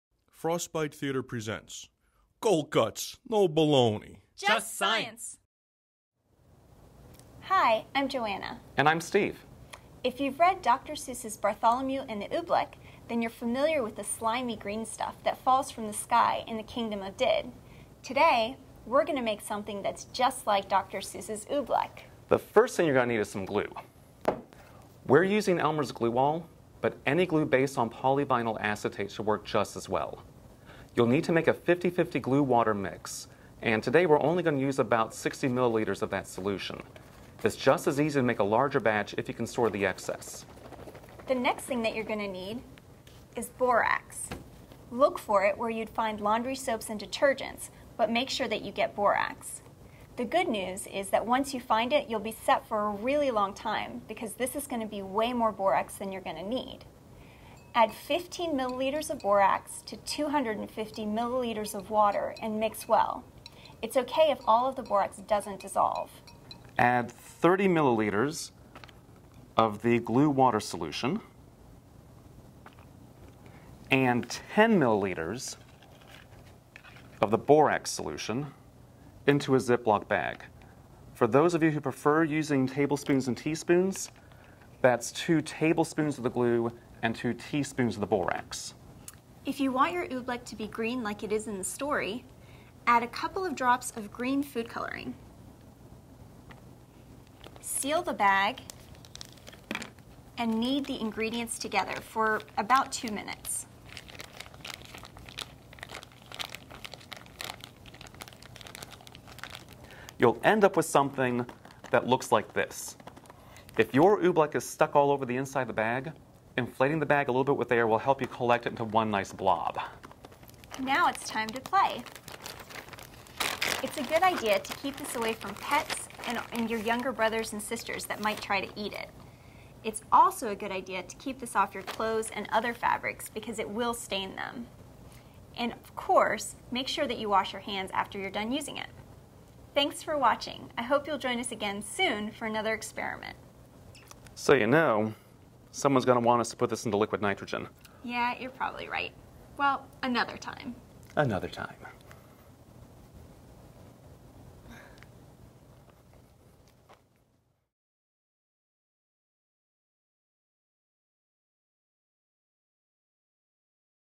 Let's Make Oobleck!
I bleck, you bleck, we all bleck for Oobleck! Just in time for Halloween! Joanna and Steve show you how to make your very own version of the slimy, green material described in Dr. Seuss' 'Bartholomew and the Oobleck'!